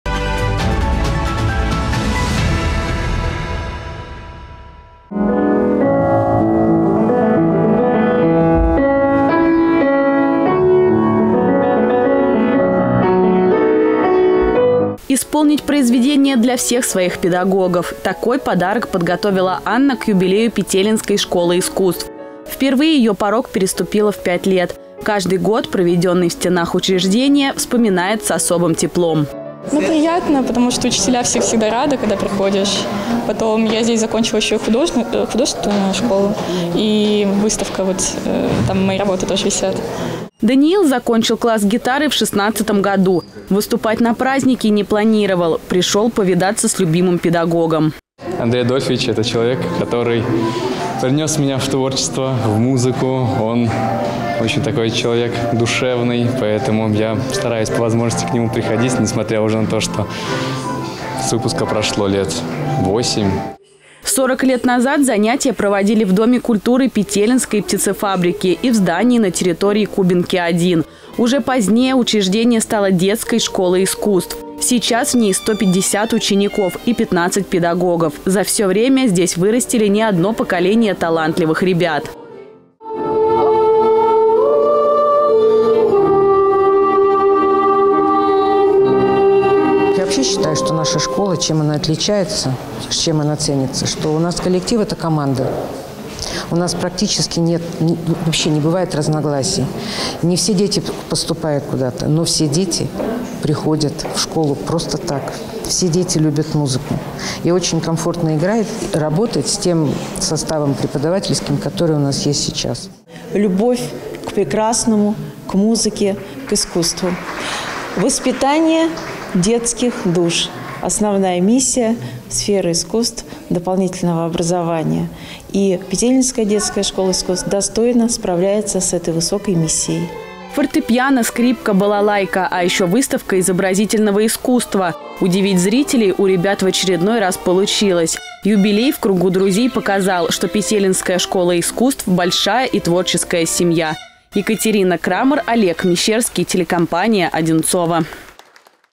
Петелинской ДШИ – 40 лет
Петелинской детской школе искусств – 40 лет. Выпускники, педагоги и учащиеся подготовили праздничный концерт - «Юбилей в кругу друзей».